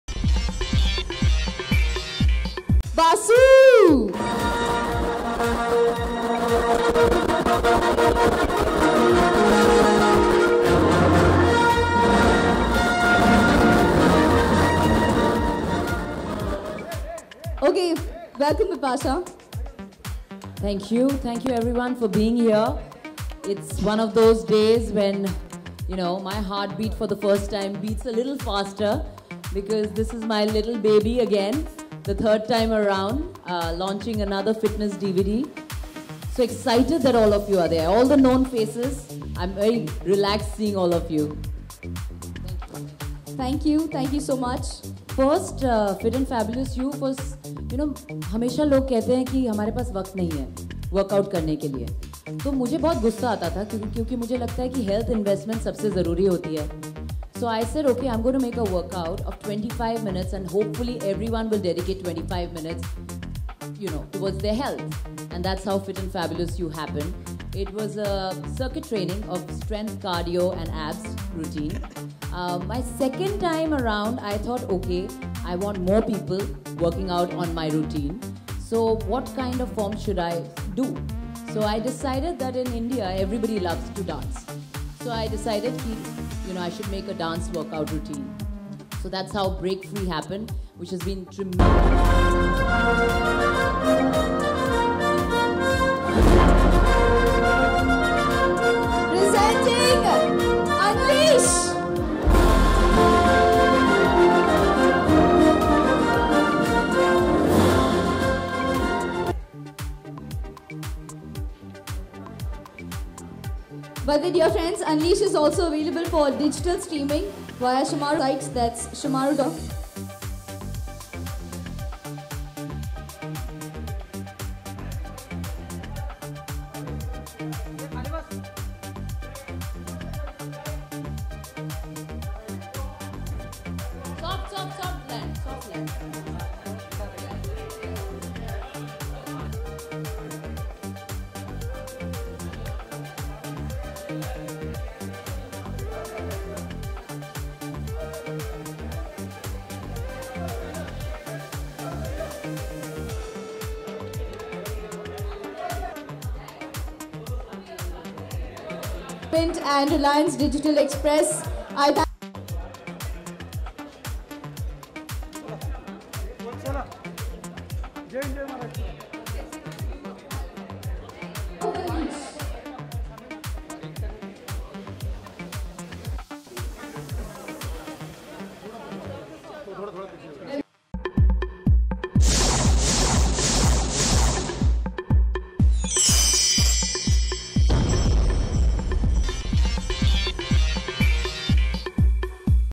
Bipasha Basu Fitness DVD - UNLEASH
Bipasha Basu Back Again With Her  Fitness DVD -- UNLEASH
Bipasha Basu with her perfect chiseled and enviably toned body has become the fitness icon of the industry. Her series of fitness DVDs -- Fit & Fabulous You and Break Free have achieved major success in the genre. Now she is ready with her 3rd DVD in the series -- Unleash. The fitness diva released Unleash in Reliance Digital Express, Mumbai along with Hiren Gada --Director and Atul Maru -- Jt Managing Director of Shemaroo Entertainment amid her fans. The actress also enjoyed watching the enthusiastic audience who tried the work out under the instructor's vigilance. She was seen addressing the audience on how it is essential to stay fit and also be strong,
Bipasha Basu Share her feelings on her latest fitness DVD Unleash "I am very excited to launch the 3rd DVD edition "Unleash "under my BBLYS series. It is fantastic routine with great benefits. Is also helps you build up your stamina and endurance levels. And surely a lot of weight loss happens, and also you become strong with this routine.
Bipasha Basu said ' She have a routine daily one hour to do the exercise.